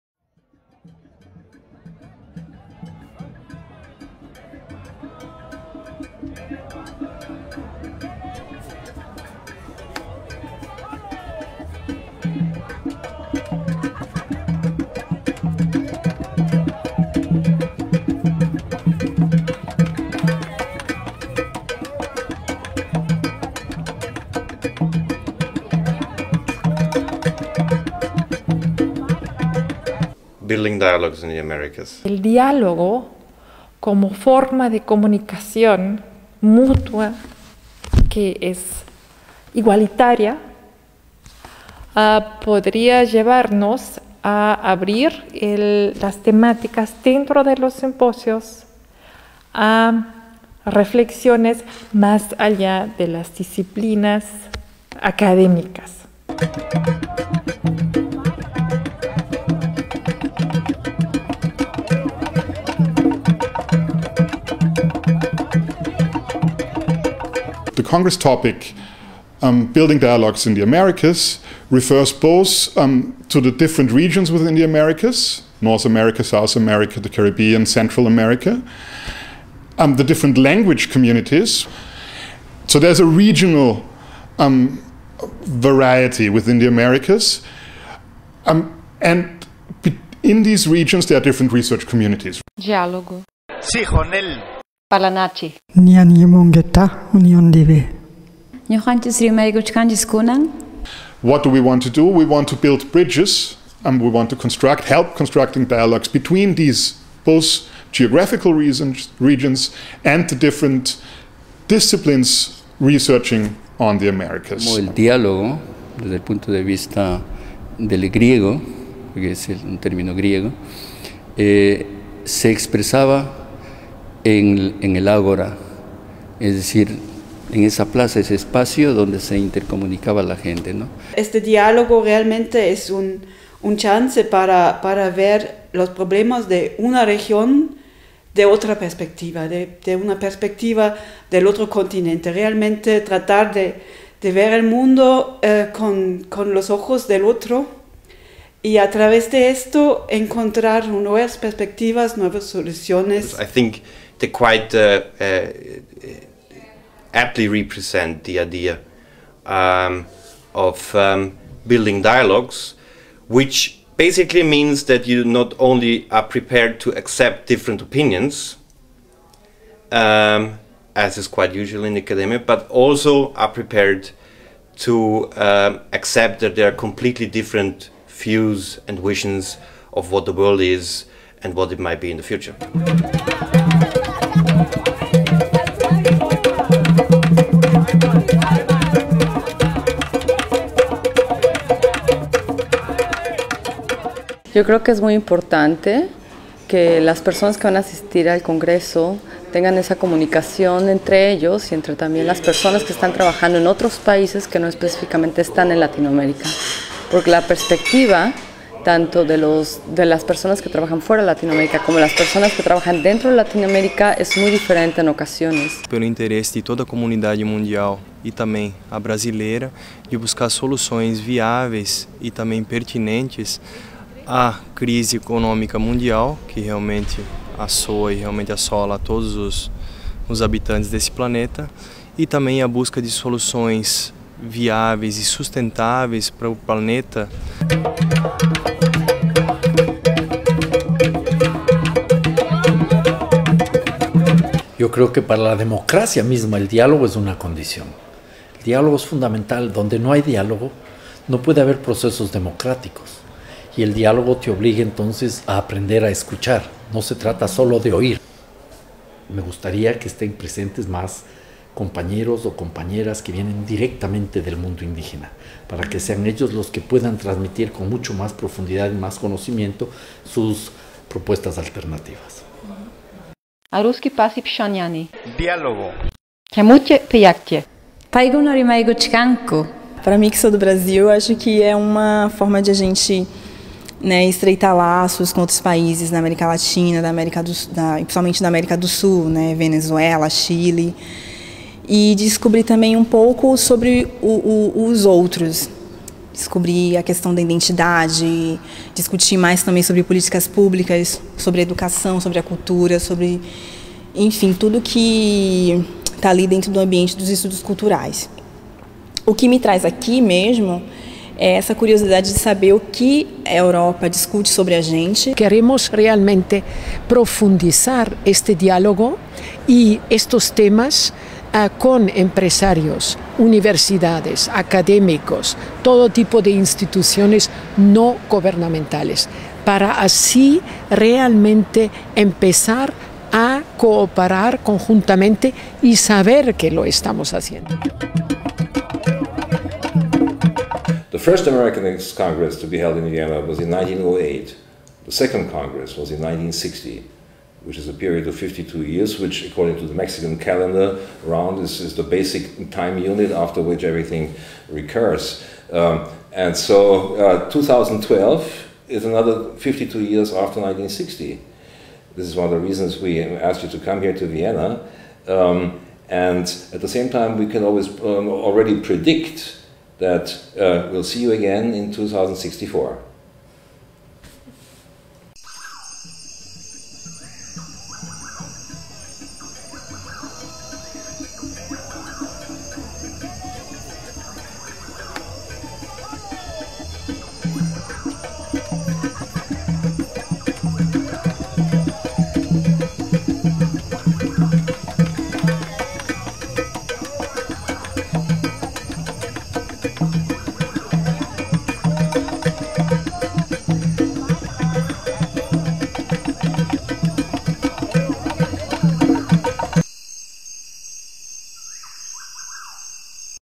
Construyendo Dialogues nas Américas
Video zum 54. International Congress of Americanists in Wien vom 15.Juli-
20.Juli 2012. Dieses Video von Eliana Pegorim und Heike Schütz-Krammer entstand
im Rahmen des Seminars zum ICA des Interdisziplinären Universitätslehrgangs
für Höhere Lateinamerikastudien des LAI Wien und der Universität Wien im
Sommersemester 2012.

Das zentrale Thema des Video ist die Darstellung der Vielfalt der „Amerikas", welches
einerseits in verschiedenen Interviews zum Kongressthema „Building Dialogues in the
Americas" und andererseits mit Sprachbildern der unterschiedlichen Sprachen, die in
den „Amerikas" gesprochen werden, zum Ausdruck kommt.

Gesprächspartner sind: Alberto Acosta, Deise de Brito, Rossana Castro Landeros,
Segundo E. Moreno Yanéz, Christian Feest, Benita Ferrero-Waldner, Martina Kaller,
David Mayer, Berthold Molden, Haroldo Ramthun Jr. und Stefanie Reinberg

Video concerning the 54th International Congress of Americanists in Vienna
from July 15-July 20, 2012. This video by Eliana Pegorim and Heike Schütz-Krammer
was developed as part of the seminar to the ICA of the Interdisciplinary Master
Degree Programme in Latin-American Studies of the Latin American Institute of
Vienna and the University of Vienna in the summer semester 2012.

The central theme of the video is to show the diversity of the "Americas", on the
one hand in several interviews on the Congress theme "Building Dialogues in the
Americas" and on the other hand with speech frames of different languages spoken
in the "Americas".

Interviewees are: Alberto Acosta, Deise de Brito, Rossana Castro Landeros, Segundo
E. Moreno Yanez, Christian Feest, Benita Ferrero-Waldner, Martina Kaller, David
Mayer, Berthold Molden, Haroldo Ramthun Jr. and Stefanie Reinberg